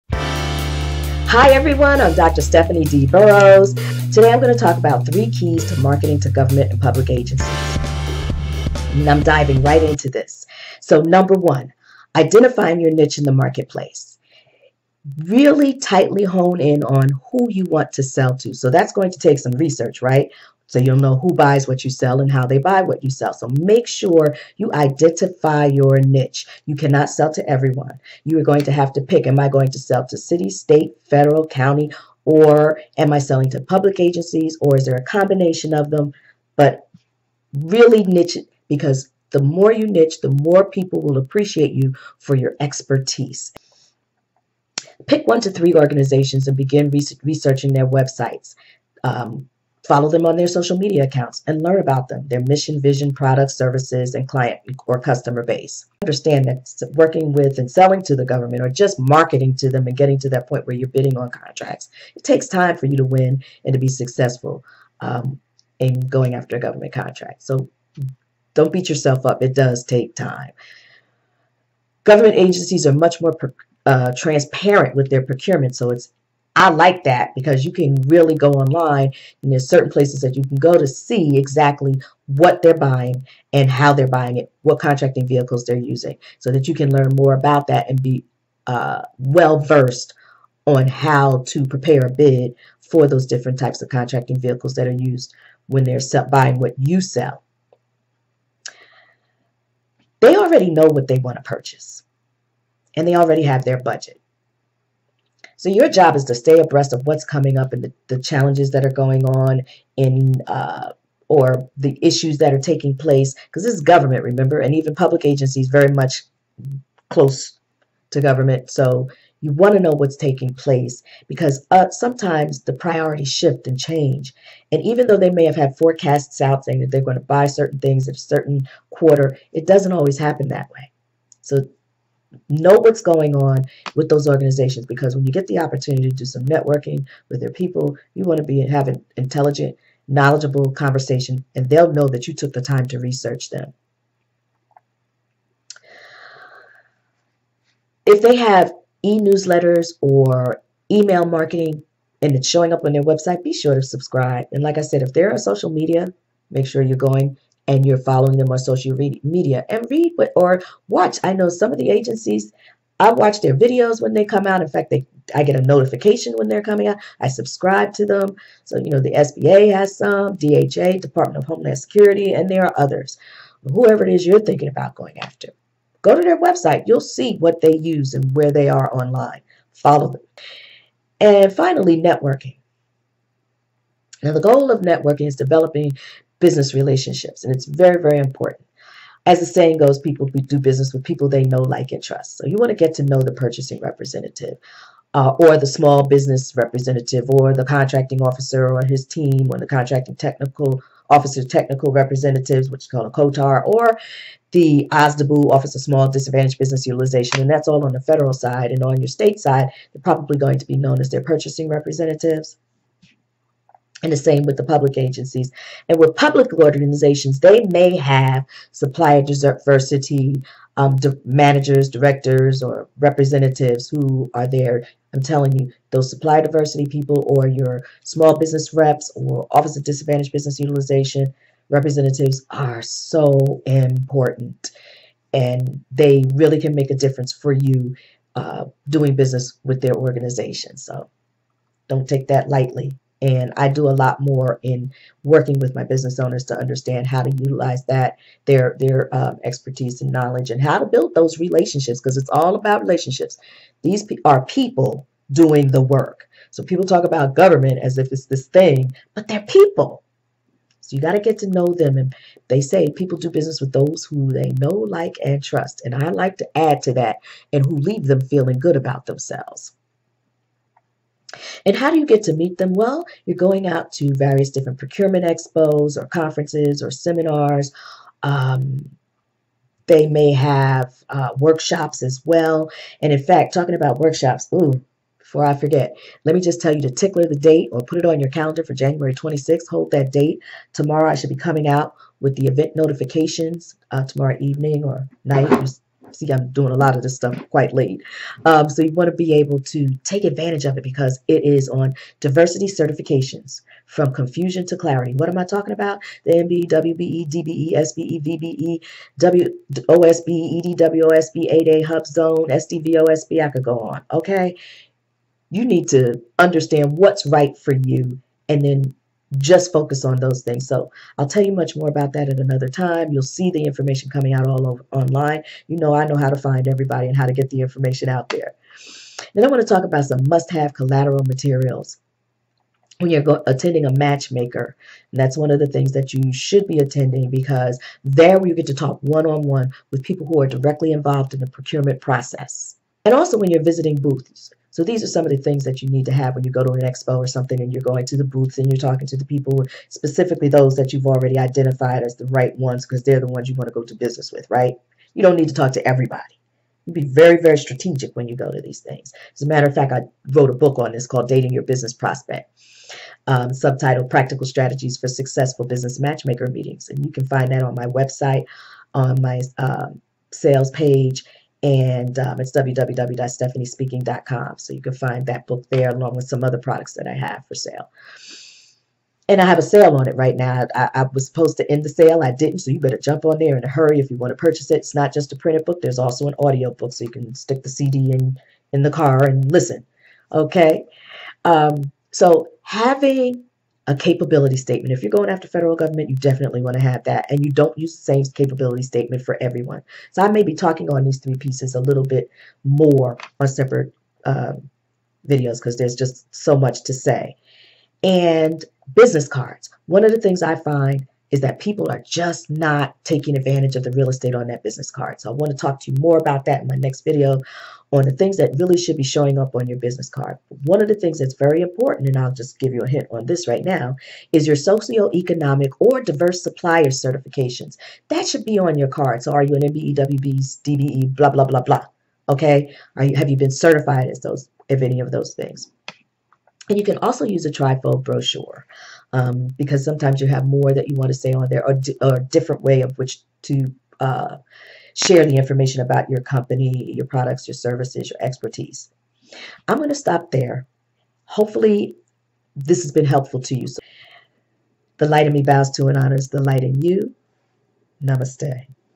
3 Keys for Marketing to Government and Public Agencies
Dr. Stephanie D. Burroughs shares quick tips on 3 areas of marketing to government and public agencies.